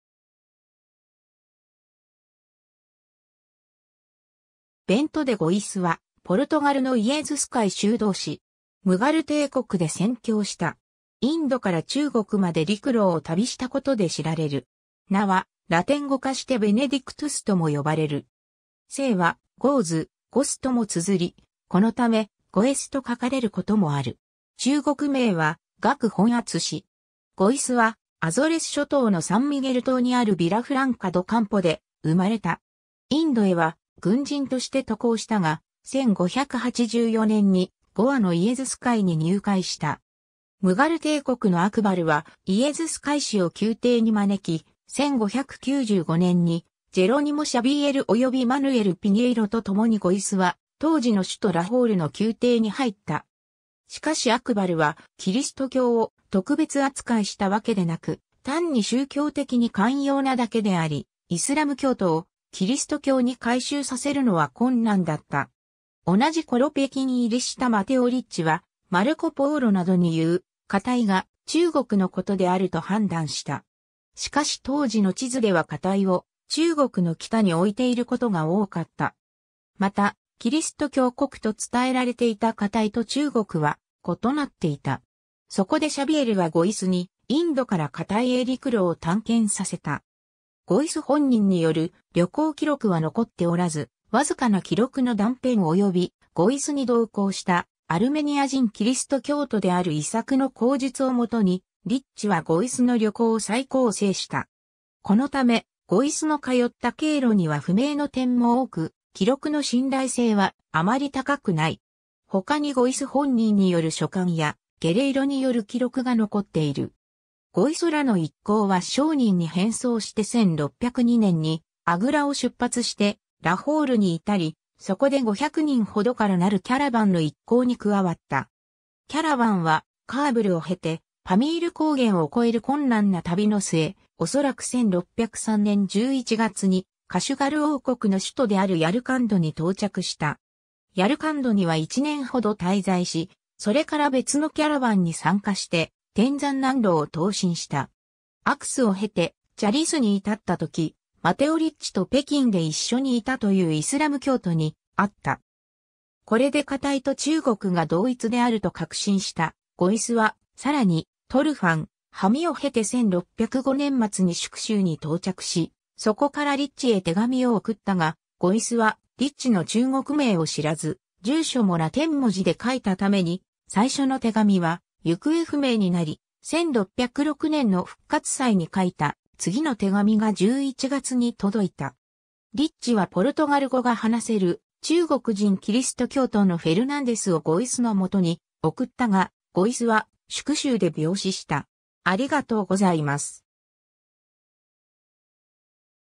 ベント・デ・ゴイス
ベント・デ・ゴイス（Bento de Góis、1562年 - 1607年4月11日）はポルトガルのイエズス会修道士。
ムガル帝国で宣教した。
インドから中国まで陸路を旅したことで知られる。
 名はラテン語化してBenedictusとも呼ばれる。
姓はGoes, Goës ともつづり、このため「ゴエス」と書かれることもある。
中国名は鄂本篤(È Běndǔ)。
 ゴイスはアゾレス諸島のサンミゲル島にあるヴィラ・フランカ・ド・カンポで生まれた。
インドへは軍人として渡航したが、1584年にゴアのイエズス会に入会した。
 ムガル帝国のアクバルはイエズス会士を宮廷に招き、1595年にジェロニモ・シャヴィエルおよびマヌエル・ピニェイロとともにゴイスは当時の首都ラホールの宮廷に入った。
しかしアクバルはキリスト教を特別扱いしたわけでなく単に宗教的に寛容なだけであり、イスラム教徒をキリスト教に改宗させるのは困難だった。
 同じ頃北京入りしたマテオ・リッチはマルコ・ポーロなどに言う「カタイ」が中国のことであると判断した。
しかし当時の地図ではカタイを中国の北に置いていることが多かった。
また、キリスト教国と伝えられていたカタイと中国は異なっていた。
そこでシャヴィエルはゴイスにインドからカタイへ陸路を探検させた。
 ゴイス本人による旅行記録は残っておらず、わずかな記録の断片およびゴイスに同行したアルメニア人キリスト教徒であるイサクの口述を元にリッチはゴイスの旅行を再構成した。
このためゴイスの通った経路には不明の点も多く、記録の信頼性はあまり高くない。
ほかにゴイス本人による書簡や、ゲレイロによる記録が残っている。
 ゴイスらの一行は商人に変装して1602年にアグラを出発してラホールに至り、そこで500人ほどからなるキャラバンの一行に加わった。
キャラバンはカーブルを経てパミール高原を越える困難な旅の末、おそらく1603年11月にカシュガル王国の首都であるヤルカンドに到着した。
ヤルカンドには1年ほど滞在し、それから別のキャラバンに参加して天山南路を東進した。
アクスを経てチャリス（焉耆）に至ったとき、マテオ・リッチと北京で一緒にいたというイスラム教徒に会った。
これでカタイと中国が同一であると確信したゴイスは、さらにトルファン、ハミを経て1605年末に粛州に到着し、そこからリッチへ手紙を送ったが、ゴイスはリッチの中国名を知らず、住所もラテン文字で書いたために最初の手紙は行方不明になり、1606年の復活祭に書いた次の手紙が11月に届いた。
リッチはポルトガル語が話せる中国人キリスト教徒のフェルナンデス（鍾鳴礼）をゴイスのもとに送ったが、ゴイスは粛州で病死した。